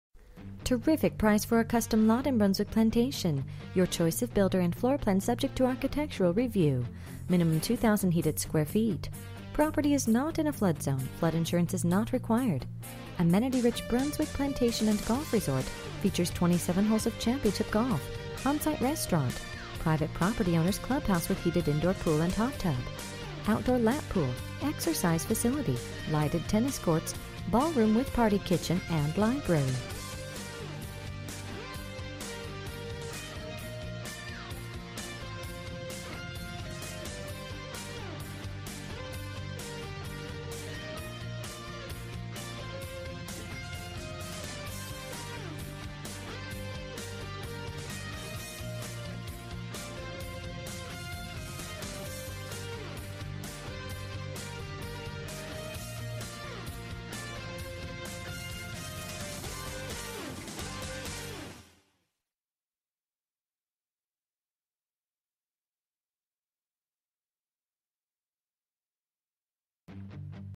Homes for sale - 1212 N Middleton Drive NW, Calabash, NC 28467
Terrific Price for a Custom Lot in Brunswick Plantation. Your Choice of Builder and Floorplan Subject to Architectural Review. Minimum 2000 Heated Square Feet. Property is NOT in a Flood Zone. flood Insurance is NOT Required Amenity Rich Brunswick Plantation and Golf Resort Features 27 Holes of Championship Golf, On-Site Restaurant, Private property Owners Clubhouse with Heated Indoor Pool and Hot Tub, Outdoor Lap Pool, Exercise Facility, Lighted Tennis Courts, Ballroom with Party Kitchen and Library.

Bedrooms: 0
Bathrooms: 0
Square Feet: 0
Price: $29,000

03/21/2016 09:47:01 am

Last modified: